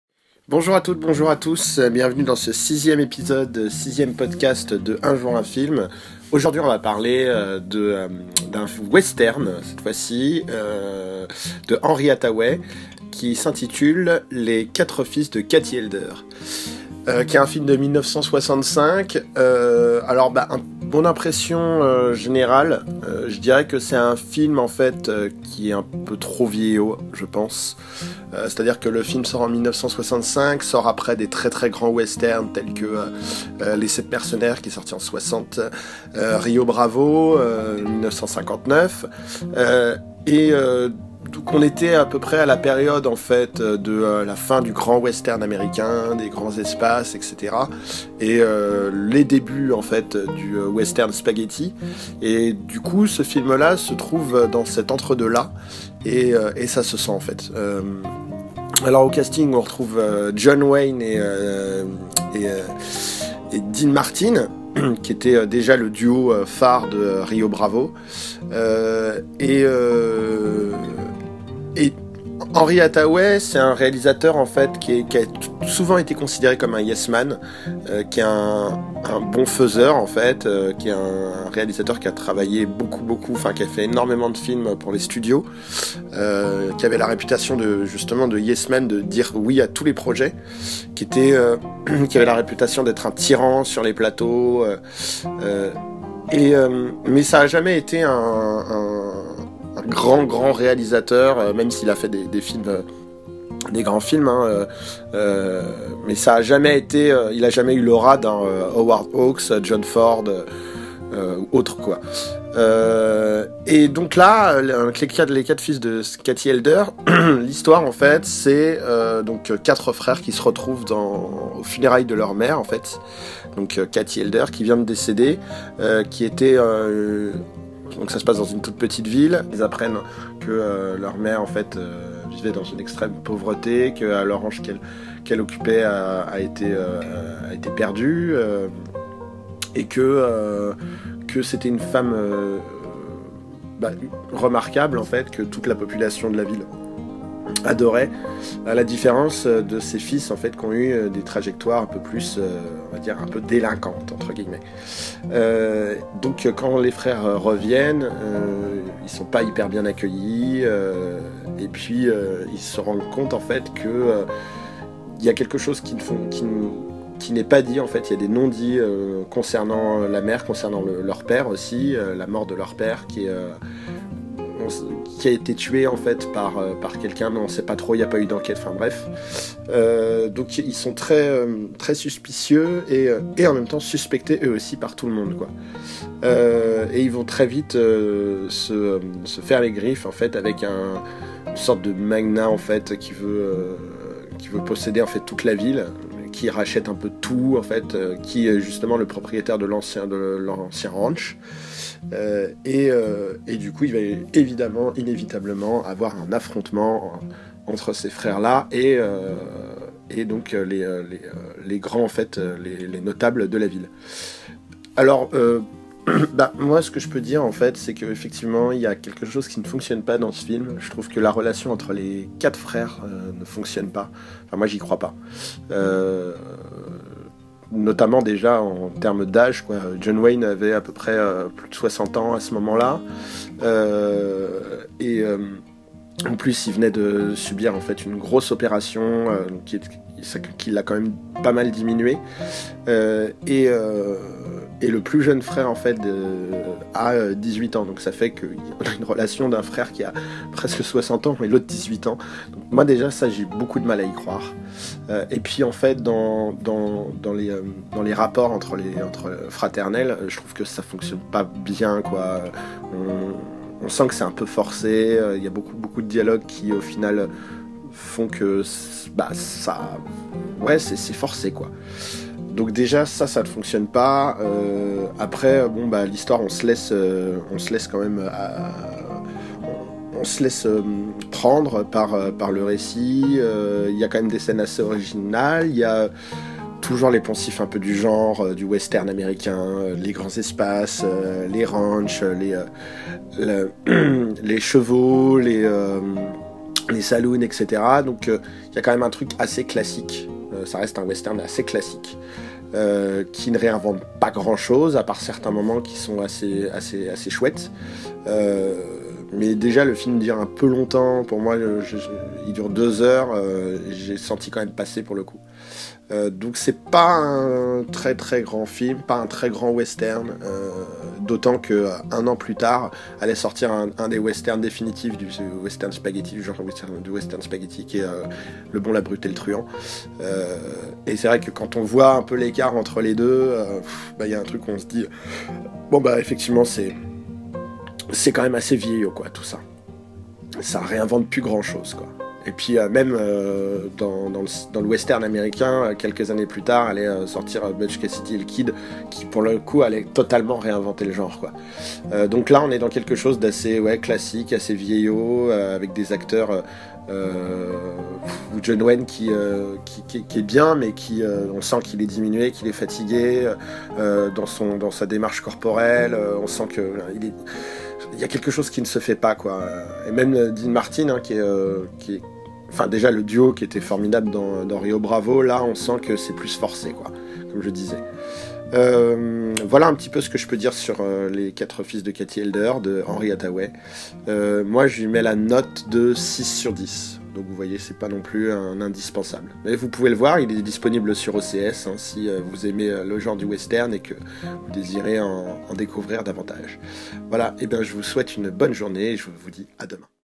UN JOUR, UN FILM - LES 4 FILS DE KATIE ELDER #6
John Wayne et Dean Martin réunis quelques années après Rio Bravo pour un wester un peu vieillot signé Henry Hathaway. Loin d'être un grand film malgré quelques bonnes scènes et une superbe musique de Elmer Bernstein. Disponible sur OCS.